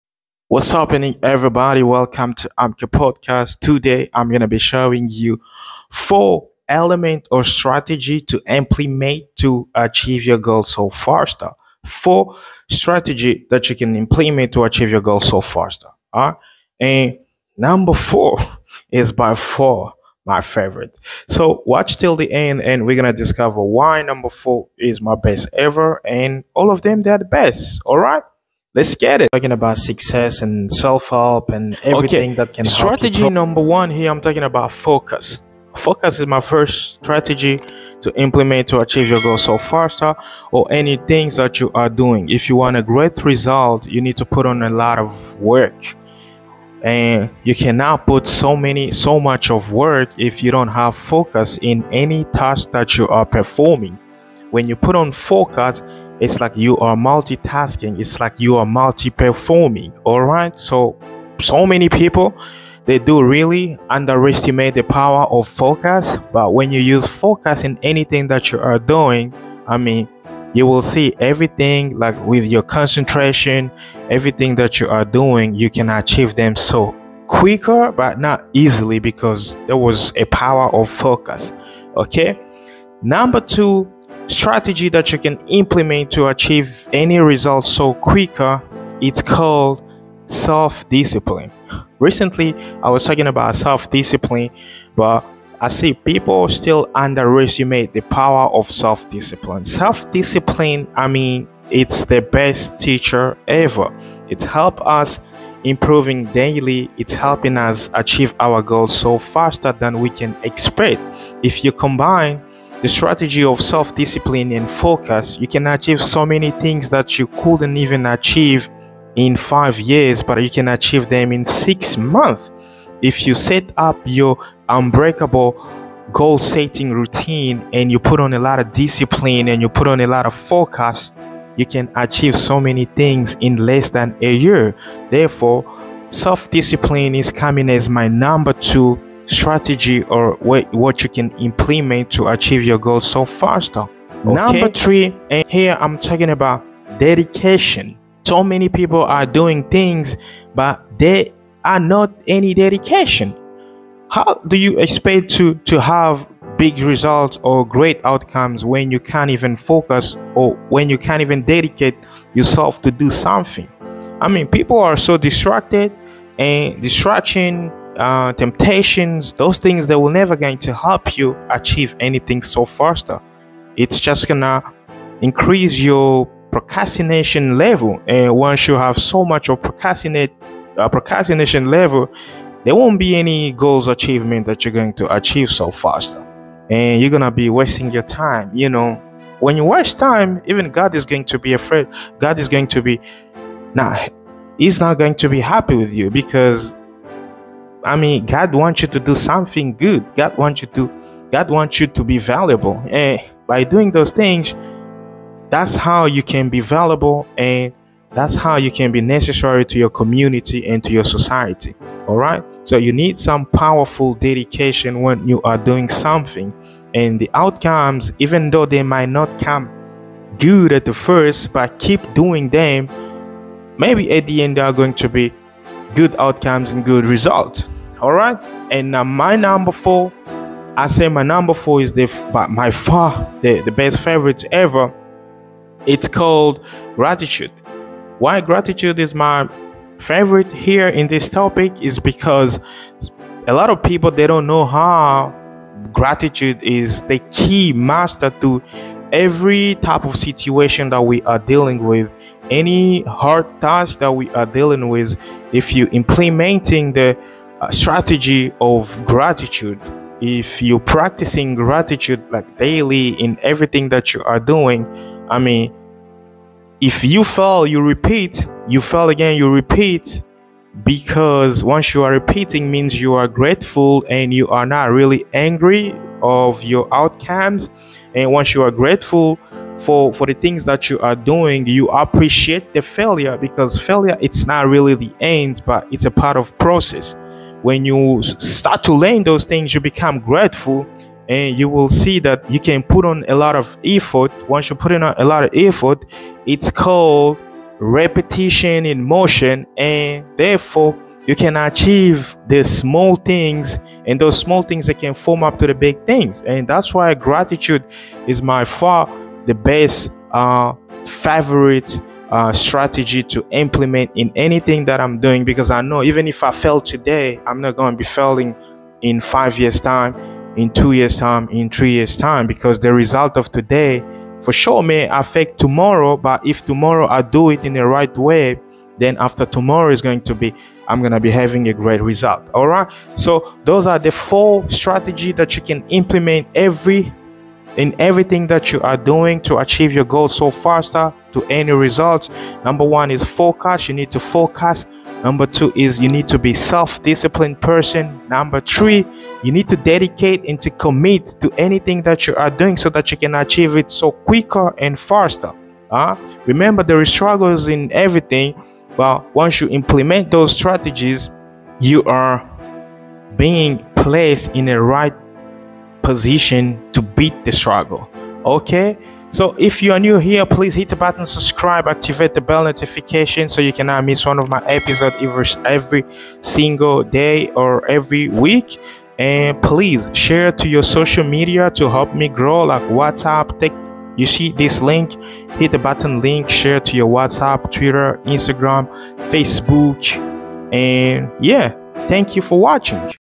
4 Strategies to achieve your goals faster (how to reach your goals)
4 strategies to achieve your goals faster,
- Focus : without  focus is the key master of every achievements when you are not getting tired and put on alot focus you can achieve more .
- Self-discipline  : every accomplishments needs Self-discipline  habits
 in order for it to work, that's why Self-discipline is well recommended for our goals achievement strategies, it been prove scientifically that with discipline in someone's career always take them far and mostly successful.

-  Dedication  : when you are talking about achieving something your mind is always thinking about how hard it's gonna be,
therefore when you are commit and dedicate yourself to the task you are doing you'll be already achieve it by 30%
and this is the power of being dedicated to something.

- Gratitude : Scientifically and biblically it has been proven that Gratitude is the key to our happy life and success,
when you are grateful you don't give up, you appreciate the outcomes and continue the journey and definitely if you continue you'll get to the desired point .